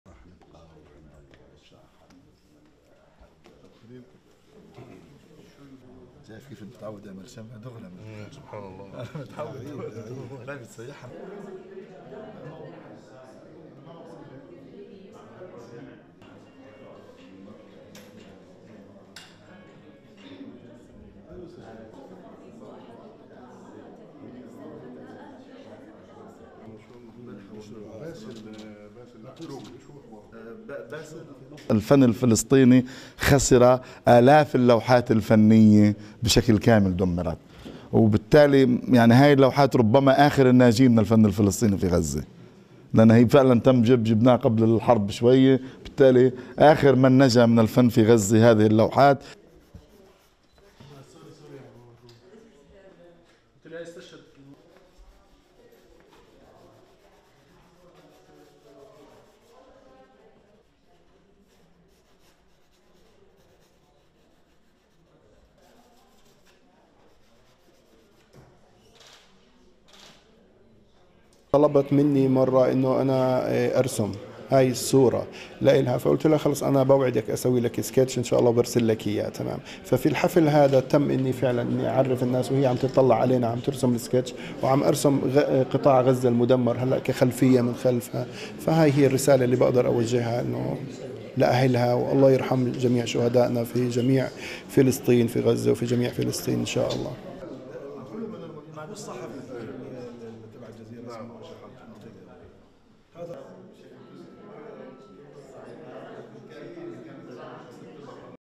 "100 Paintings from Gaza" to support Palestinian artists and their families
RESTRICTION SUMMARY:

ASSOCIATED PRESS
Ramallah, West Bank -  07 February 2024
1. Wide of Palestinian Minister of Culture, Atef Abu Saif, attending the opening of the fine art gallery "100 Paint from Gaza" in Ramallah's Mahmoud Darwish Museum
2. Tilt up of Abu Saif, Palestinian officials and artists lighting candles at the opening of the gallery for the lost of Palestinian artists during the war in Gaza
3. Various of artwork and attendants at the gallery
4. Wide of Abu Saif and Palestinian officials looking at the art
5. SOUNDBITE (Arabic) Atef Abu Saif, Minister of Culture:
"Palestinian art has lost thousands of artworks, completely destroyed, and therefore these paintings may be the last survivors of Palestinian art in Gaza. They were brought before the start of the war, and as a result, these paintings are the last surviving creations of original Palestinian artists in Gaza."
6. Various of Abdel Hadi Yaish, a Palestinian artist making a portrait of Heba Zagout in her honour
7. SOUNDBITE (Arabic) Abdel Hadi Yaish, artist:
"Once, she (late artist Heba Zagout from Gaza) asked me to draw a picture for her, so I told her, "I promise you that I will do it and send it to you." Today, on this occasion, I drew her picture as she looked at us, with the devastated Gaza Strip as the background. This is the message that I can address to her family. May Allah have mercy on all our martyrs in Gaza and throughout Palestine."
8. Various of attendees
STORYLINE:
Palestinian Minister of Culture, Atef Abu Saif, on Wednesday attended the opening of the fine art gallery "100 Paint from Gaza"  in Mahmoud Darwish Museum at the West Bank city of Ramallah.

The Palestinian Ministry of Culture collected 100 artworks from 28 fine artists from the Gaza Strip before the war between Israel and Hamas, with the intention of displaying them in art galleries abroad. However, following the outbreak of the war, numerous cultural centres, artists' homes, and painting studios were destroyed. Tragically, several artists, including fine artist Heba Zagout lost their lives.

Abu Saif considered organizing the gallery at this time as a "tribute to artists from different generations in Gaza, most of whom are in refugee camps, Rafah city and its various neighbourhoods. "Palestinian art has lost thousands of artworks, completely destroyed, and therefore these paintings may be the last survivors of Palestinian art in Gaza. They were brought before the start of the war, and as a result,  these paintings are the last surviving creations of original Palestinian artists in Gaza," he said.

The sale proceeds of the artworks will be directed to support Palestinian artists and their families, who have been affected by the Israeli war against Gaza, according to Walied Ayoub, the director of the fine art department at the Ministry of Culture.

Abdel Hadi Yaish, a Palestinian artist, sent a message to the family of the late artist Heba Zagout through a portrait of her among destroyed houses in Gaza as a symbol of his grief over her loss during the opening of the gallery.

AP Video shot by Imad Issied, produced by Jalal Bwaitel.